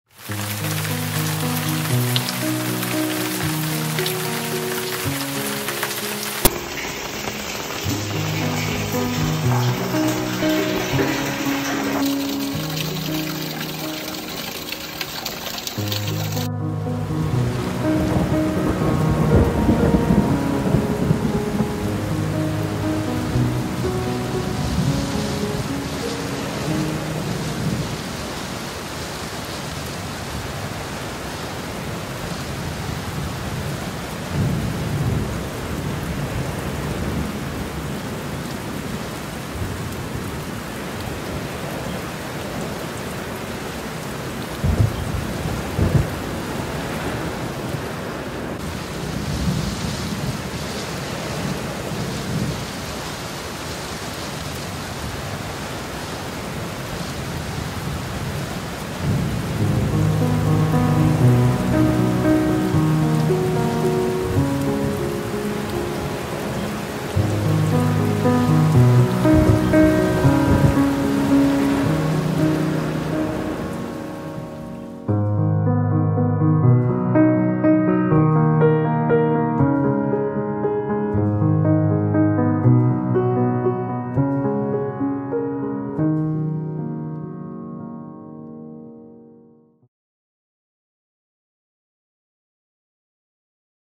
I WATCHED 4 CARS CROSS THIS FLOODED ROADWAY - Would YOU DO IT?
Never Ever Drive into or across a flooded road. I witnessed 4 cars crossing a flooded roadway that was barricaded and closed. This was dangerous and you should never attempt this. During Monsoon Season here in Arizona roadways and normally dry washes and river beds can experience Flash Flooding without any warning.